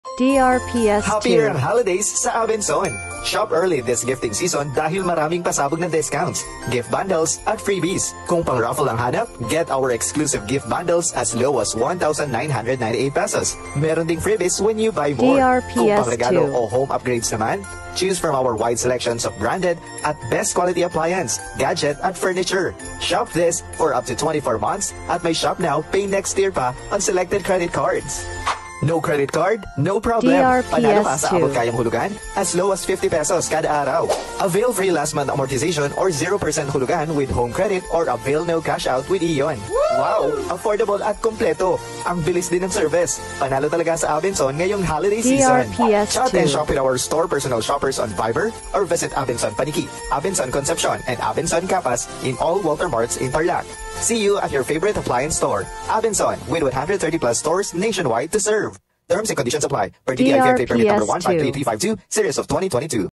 Abenson Christmas Promos Radio Ad Q4 2022 63s (Tarlac)
This is the Q4 2022 Provincial Radio Ad for Abenson Christmas Promos in Tarlac City.

DRPS Two 2873: Abenson Christmas Promos Radio Ad Q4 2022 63s (Tarlac)